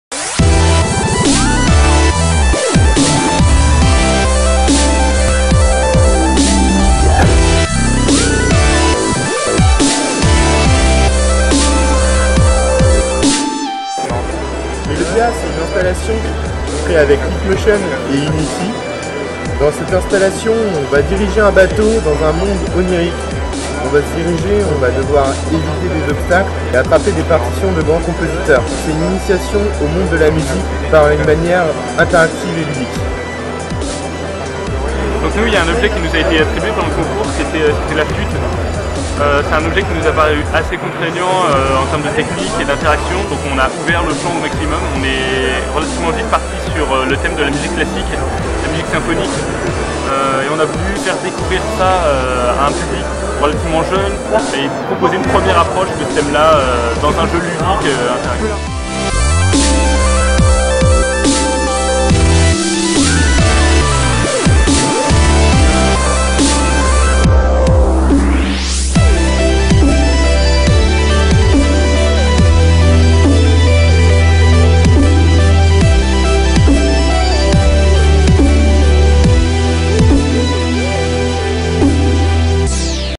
Virtual Fantasy "Limited Time" 2013 - Melodia (L'école de Design de Nantes)
Application done by the "Voxels" team from L'école de Design de Nantes (Nantes) during the competition Virtual Fantasy "Limited Time" 2013

Authors :

Lorant Vincent
Senand Benoit
Teyssier Marc
Davenas Gregoire